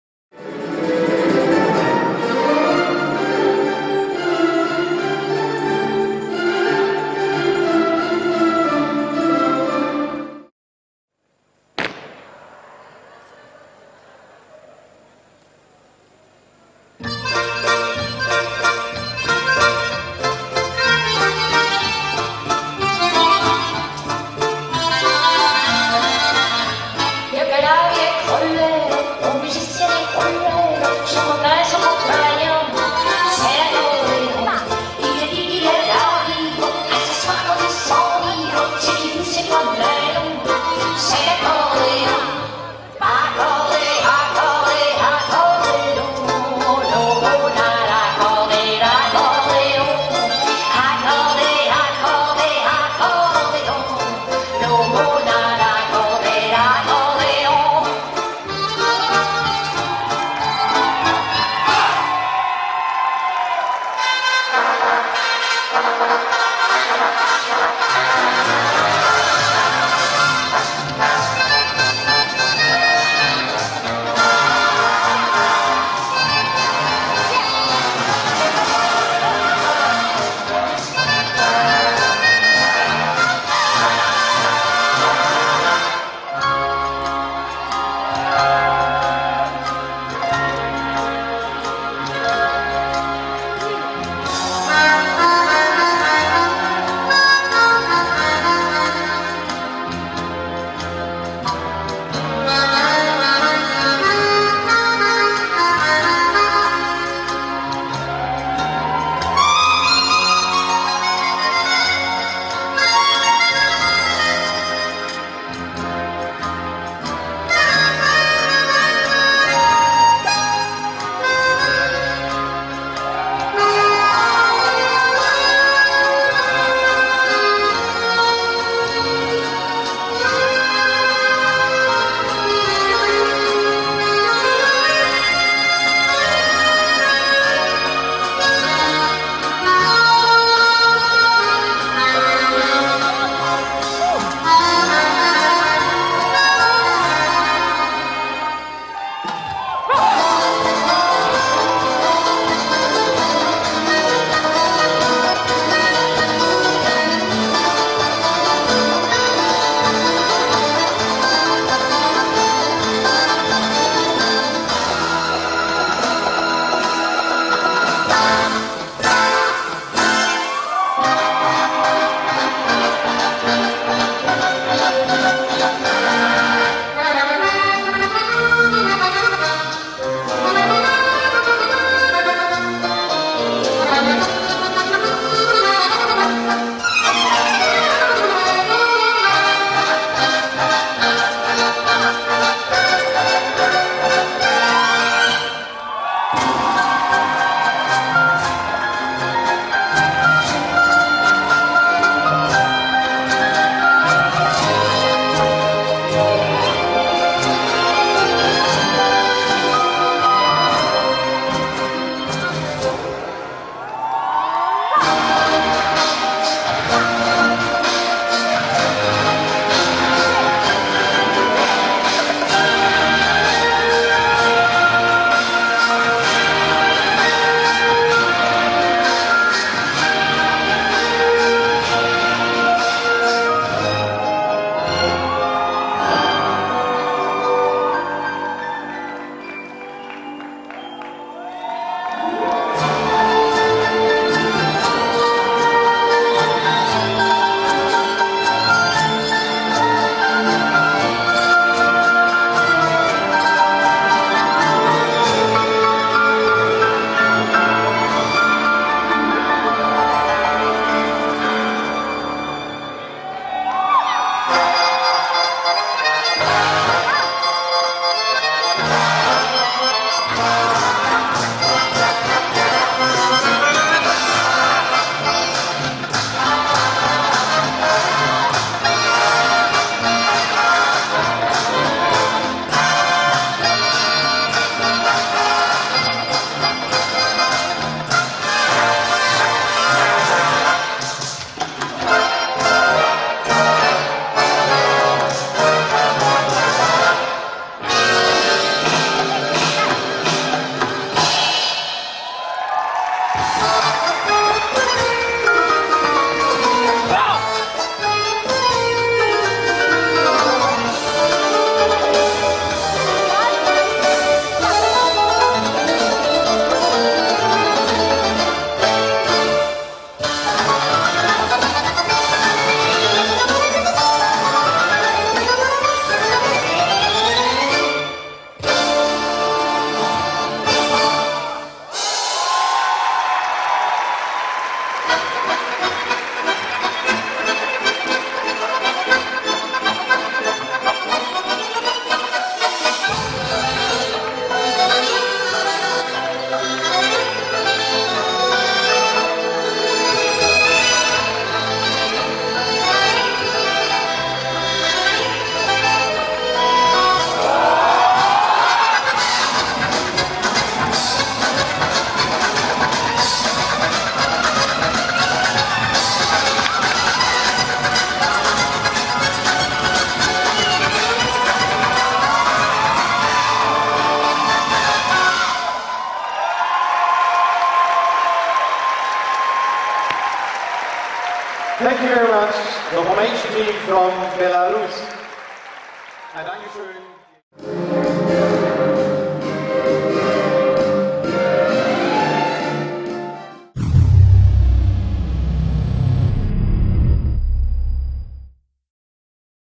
2011 World Standard Formation | Fortuna Polotsk | BLR
Team Belarus II, Fortuna Polotsk, dances its choreography in the final of the 2011 WDSF World DanceSport Championship Standard Formation. The formation placed 16th.